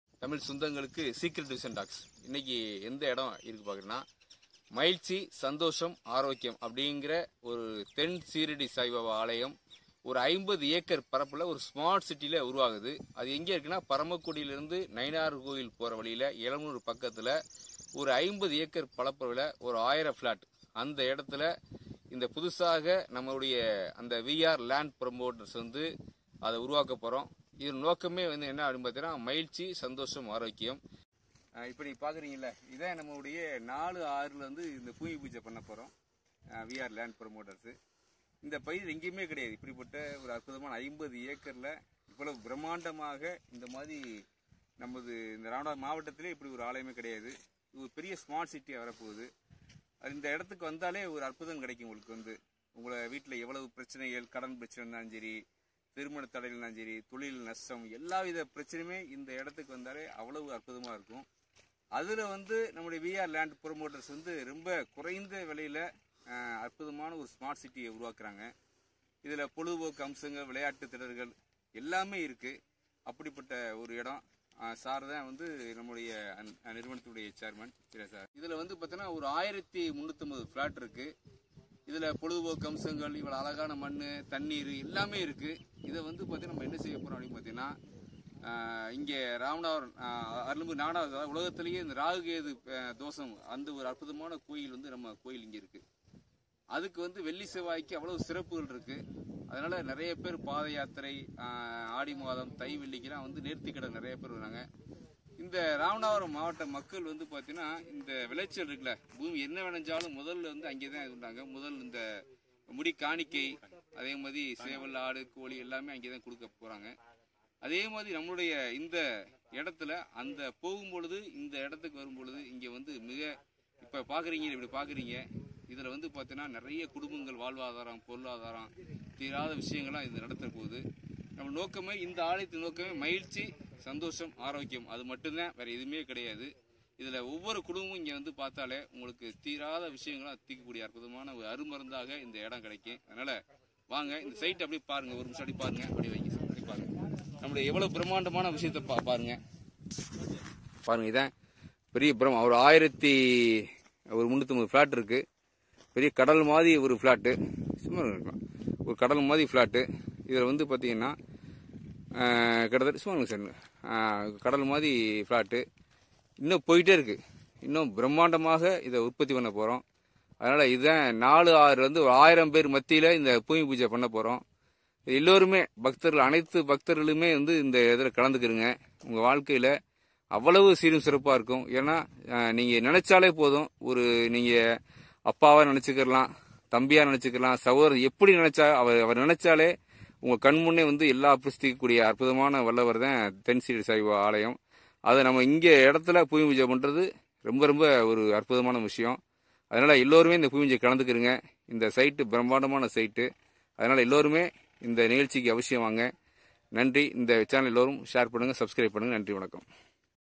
ஷீரடி பாபா கோயில் பரமக்குடி இளமனூர் அருகில் பூமிபூஜை 04/06/22
ஷீரடி பாபா கோயில் உதயமாகிறது